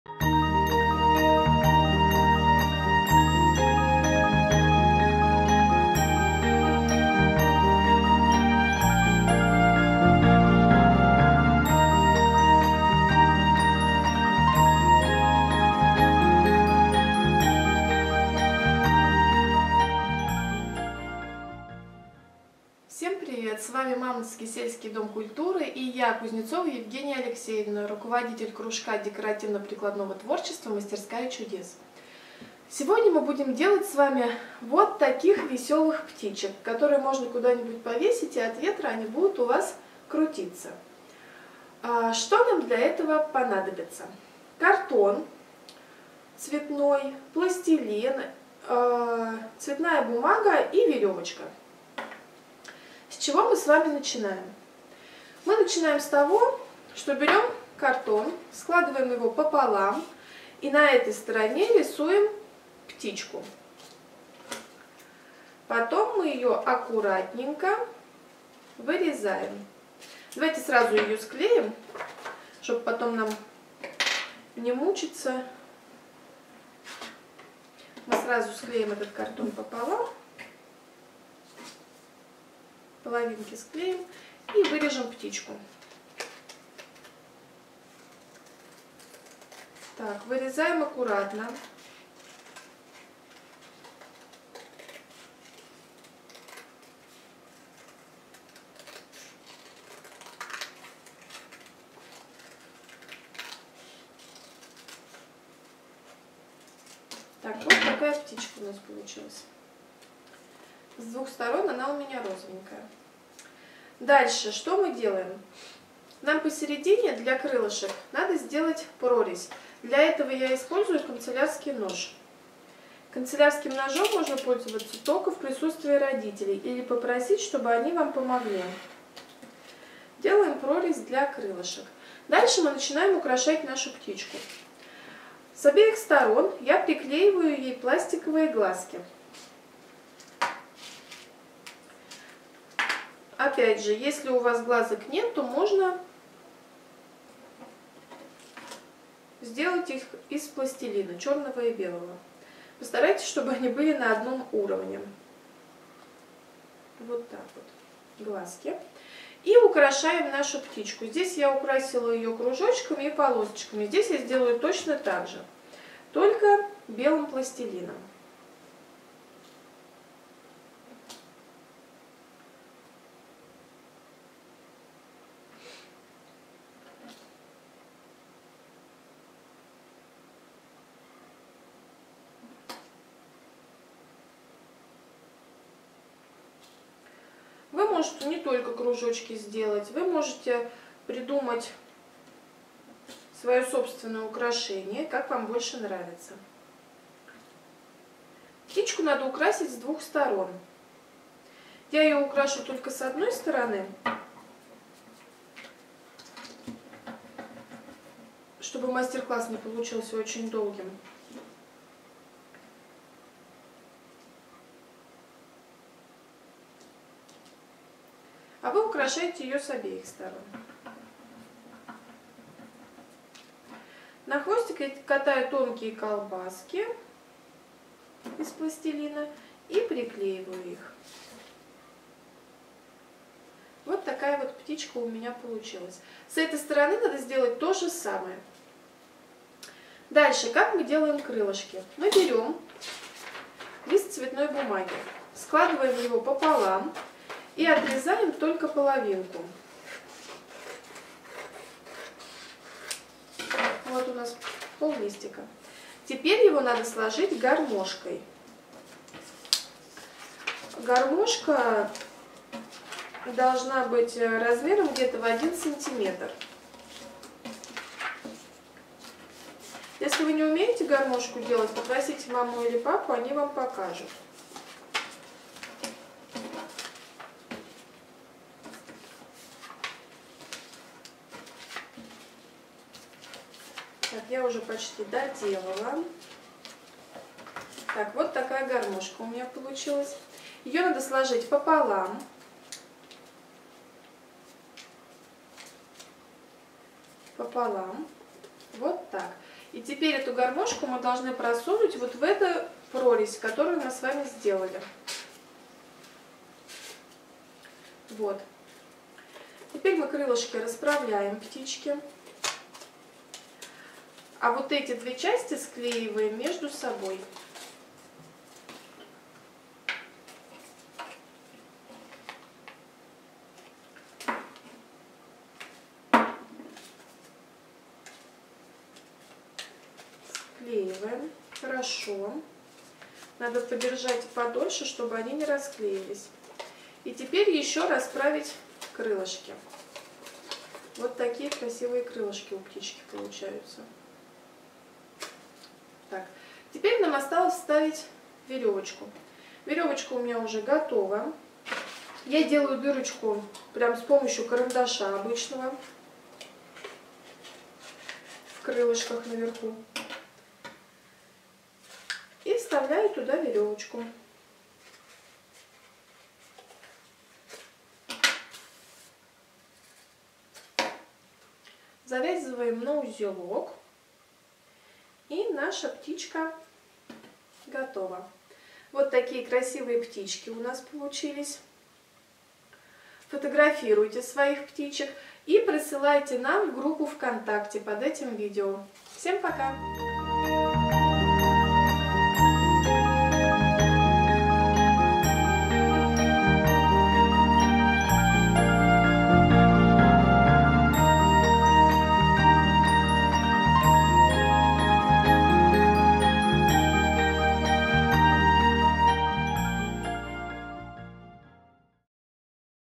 Мастерская чудес урок 7
Мастер – класс «Дружные пташки» - поделка по изготовлению веселых птичек.Ведет Евгения Алексеевна Кузнецова, преподаватель кружка прикладного творчества "Мастерская чудес".